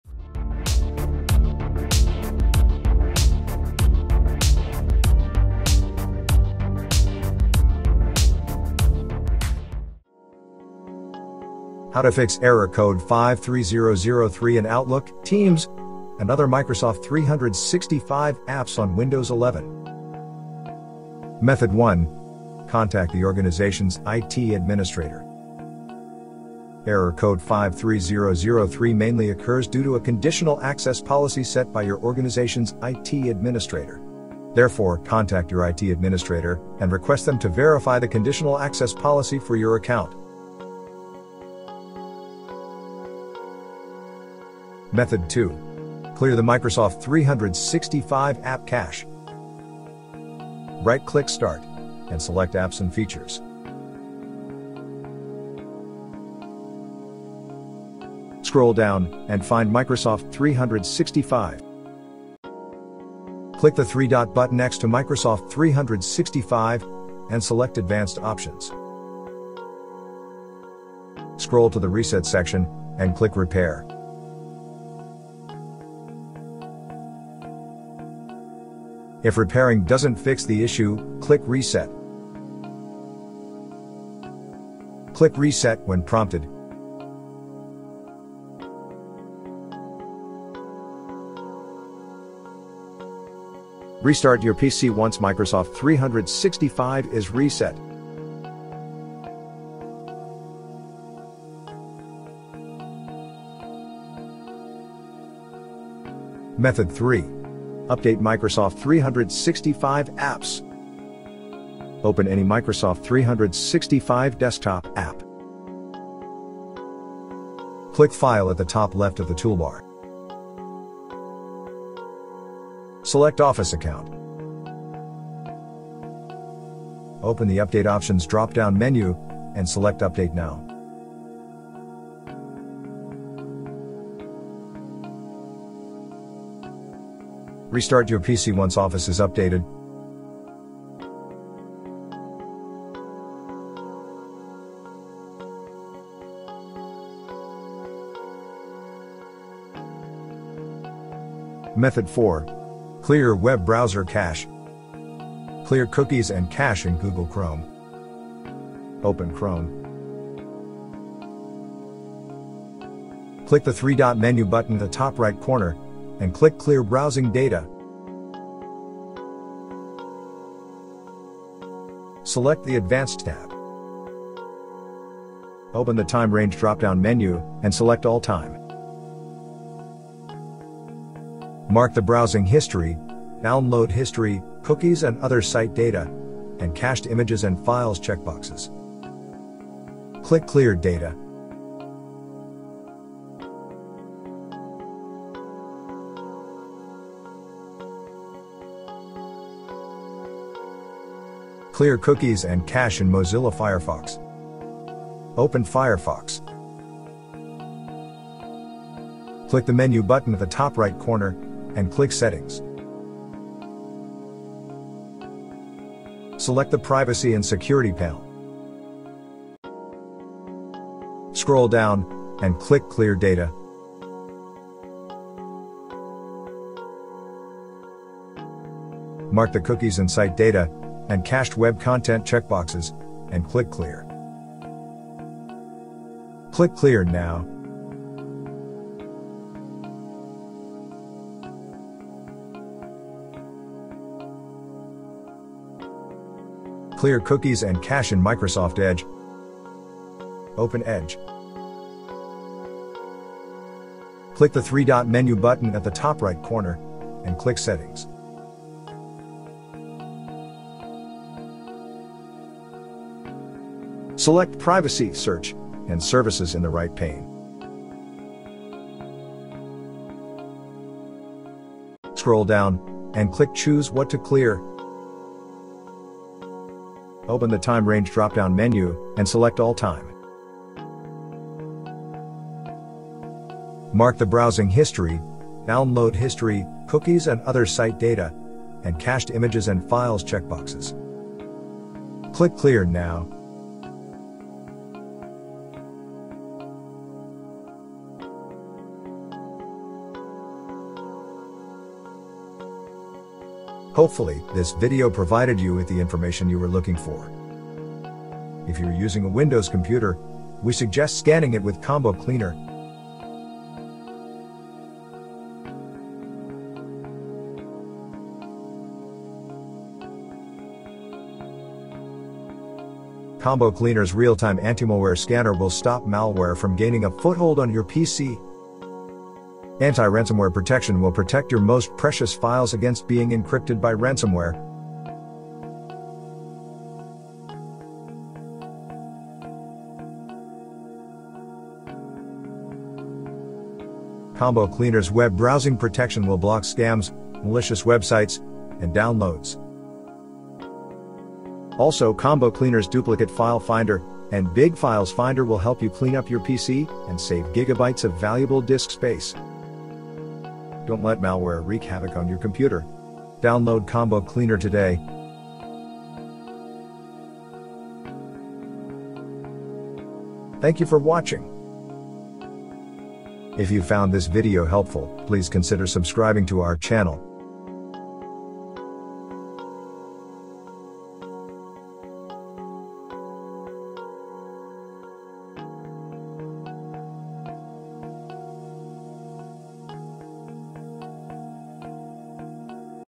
How to Fix Error Code 53003 in Outlook, Teams, and Other Microsoft 365 Apps?
Video showing how to Fix Error Code 53003 in Outlook, Teams, and Other Microsoft 365 Apps.